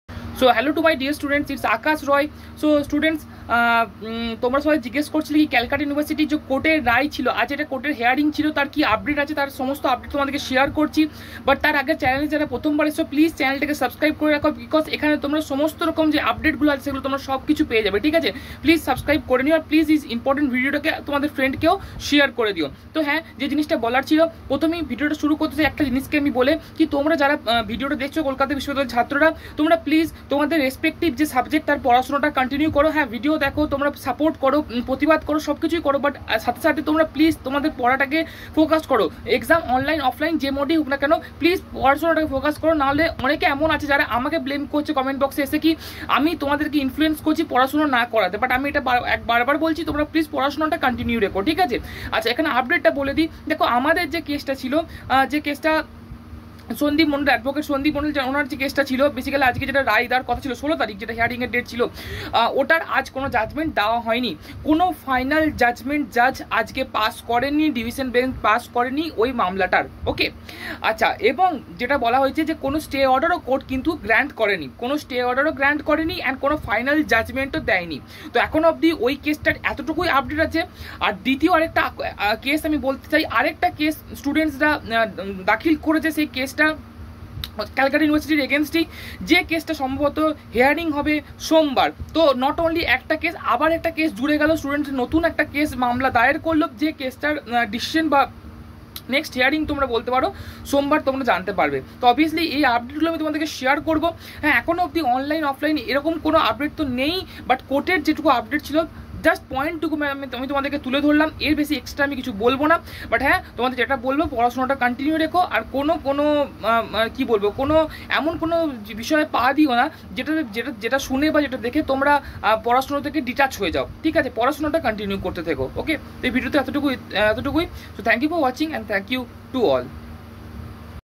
কলকাতা হাইকোর্ট কি রায় দিলো আজ | মামলার আপডেট | আবার একটি মামলা করল ছাত্ররা | CU Exam Update
1st year malda college pass course and honours merit
webscte notice
jexpo 2022
voclet
jelet 2022